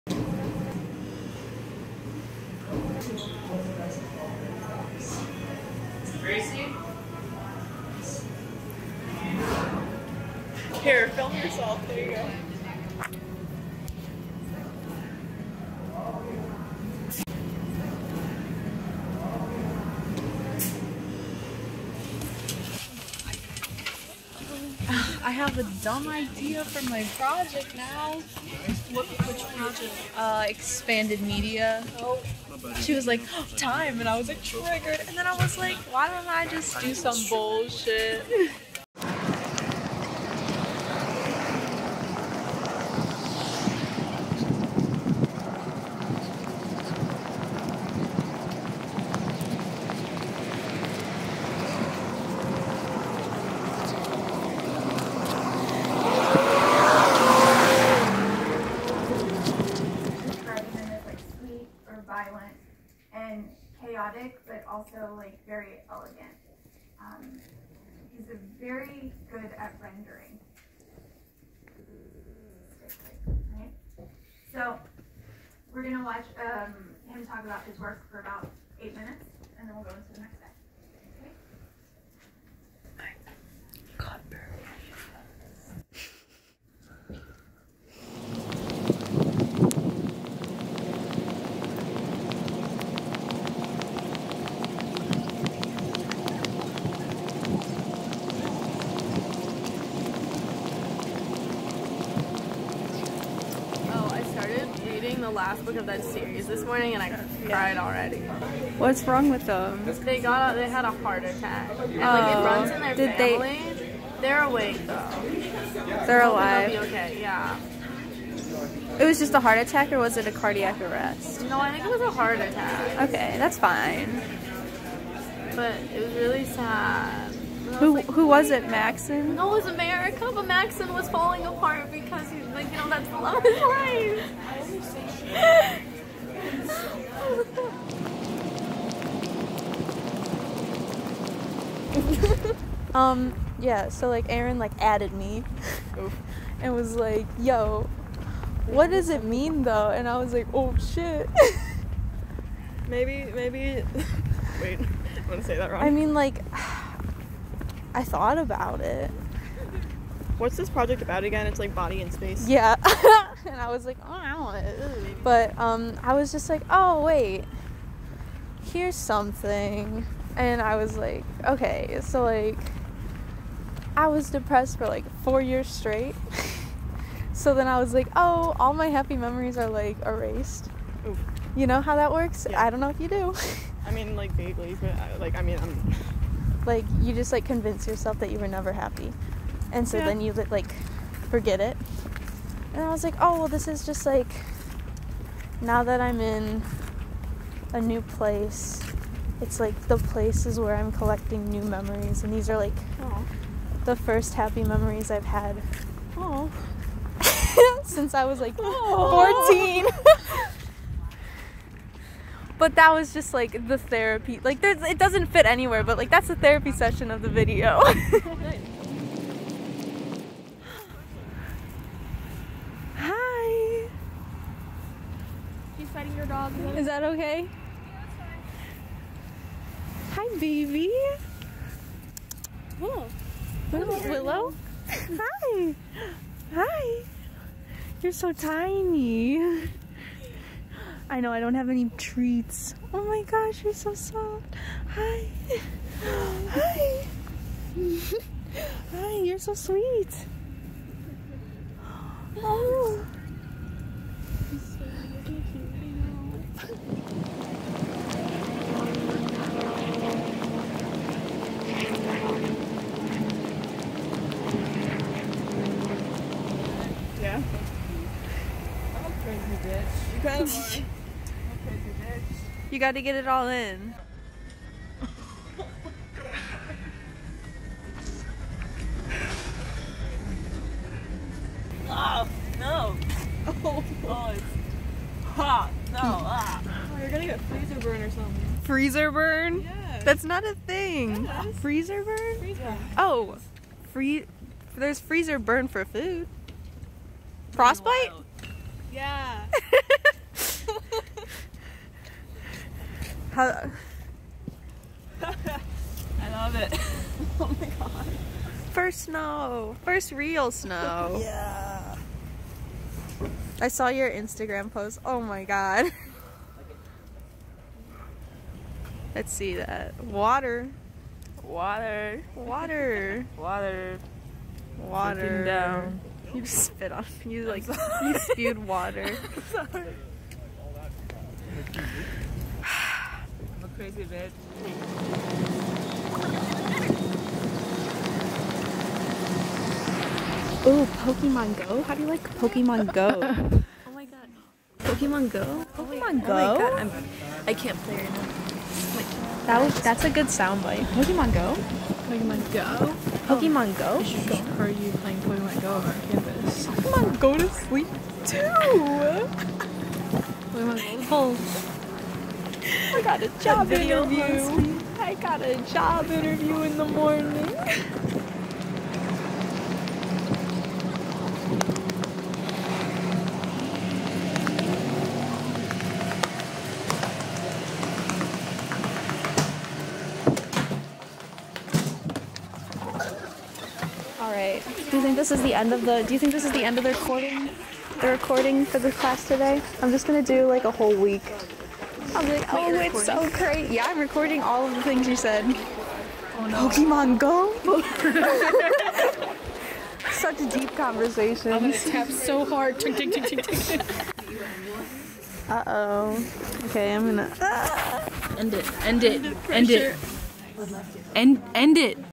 My Happy Place
Project for Expanded Media. A collection of happy memories, the first I can hold onto since my depression went away.